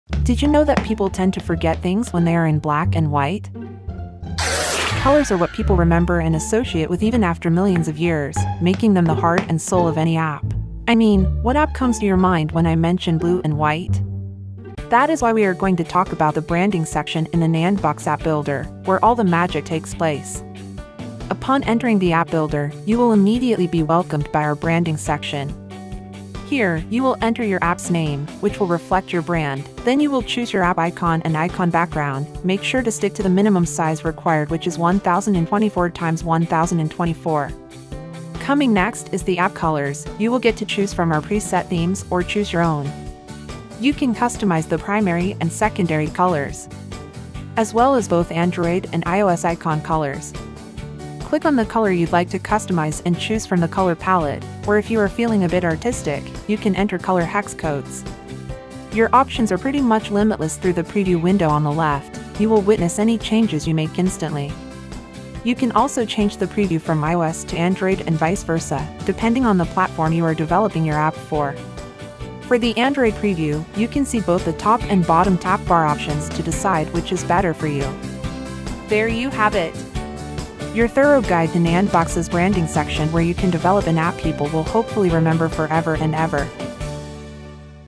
How to Build Your App? Branding Section
This video will walk you through the app branding section of the nandbox app builder. Through this section, you will easily customize your app’s colors, explore all the color palettes available, and choose an icon that will represent your app’s purpose and brand. You can do all this and even more! Now is the time to try the nandbox app builder and discover the limitless possibilities!